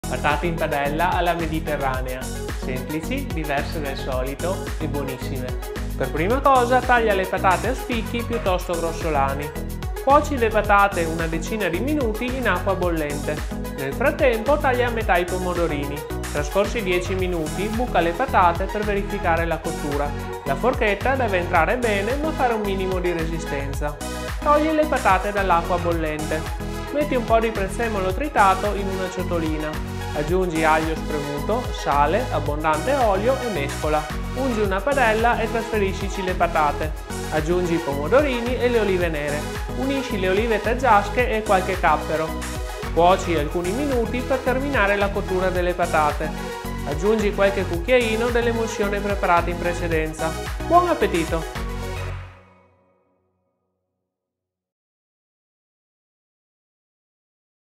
PATATE IN PADELLA ALLA MEDITERRANEA
INGREDIENTI
→ 3-4 patate
→ 300 g pomodorini
→ 2-3 cucchiai olive nere
→ 2-3 cucchiai olive taggiasche
→ 1 cucchiaio capperi dissalati
→ 1 spicchio aglio
→ sale q.b.
→ olio extravergine di oliva q.b.
→ prezzemolo q.b.

PROCEDIMENTO
Taglia le patate a spicchi piuttosto grossolani.
Cuoci le patate una decina di minuti in acqua bollente. Nel frattempo taglia a metà i pomodorini.
Trascorsi 10 minuti buca le patate per verificare la cottura. La forchetta deve entrare bene ma fare un minimo di resistenza. Togli quindi le patate dall'acqua bollente.
Metti un po' di prezzemolo tritato in una ciotolina. Aggiungi aglio spremuto, sale, abbondante olio e mescola.
Ungi una padella e trasferisci le patate. Aggiungi i pomodorini e le olive nere. Unisci le olive taggiasche e qualche cappero. Cuoci alcuni minuti per terminare la cottura delle patate.
Aggiungi qualche cucchiaino dell'emulsione preparata in precedenza. Buon appetito.